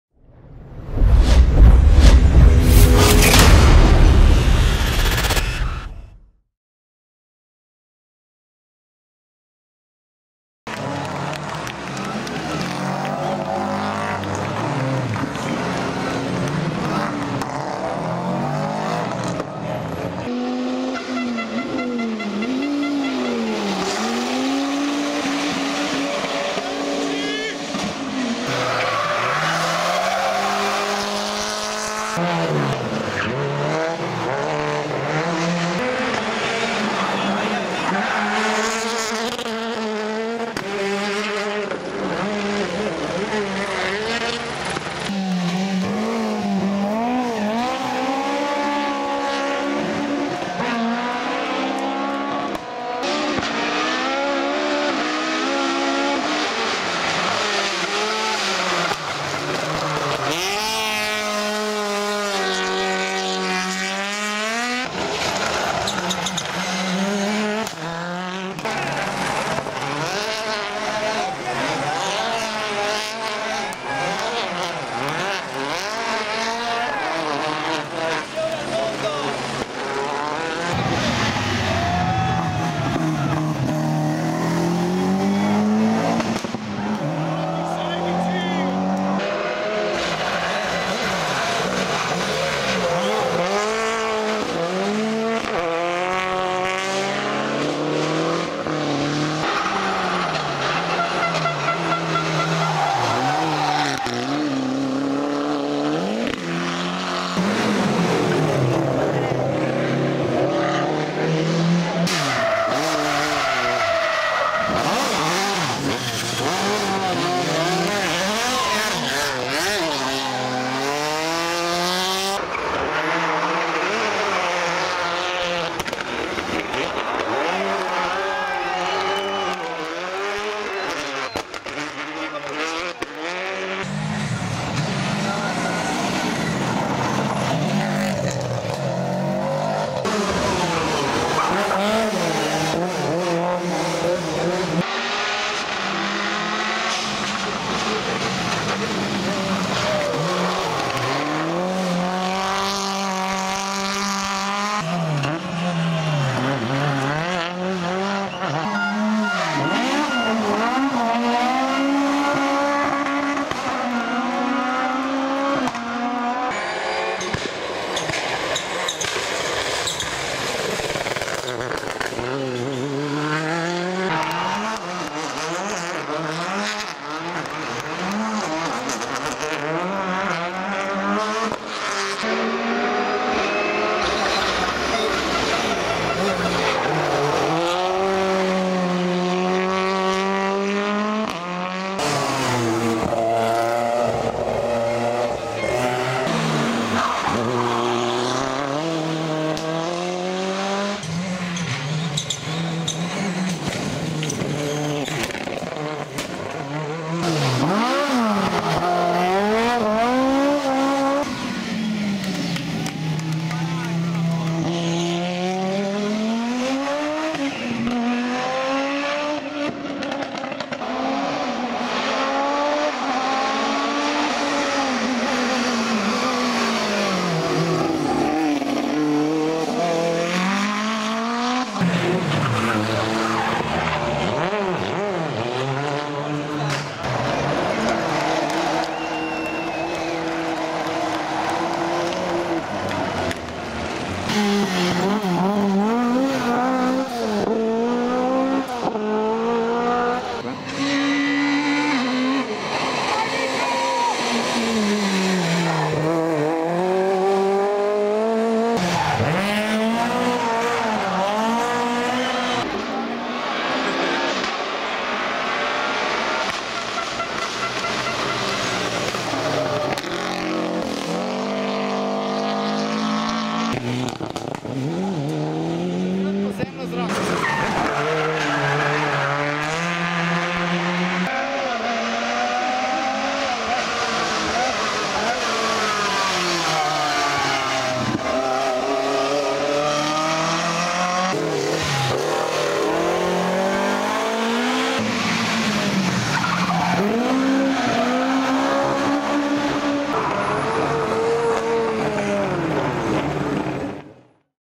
Rally Legend 2017 I ACTION and SHOW I
Highlights from the 2017 Rally Legend in San Marino. The greatest celebration of rallying with  some of the best rally cars of all times! The 2017 edition of the rally was a tribute to the beloved Colin McRae!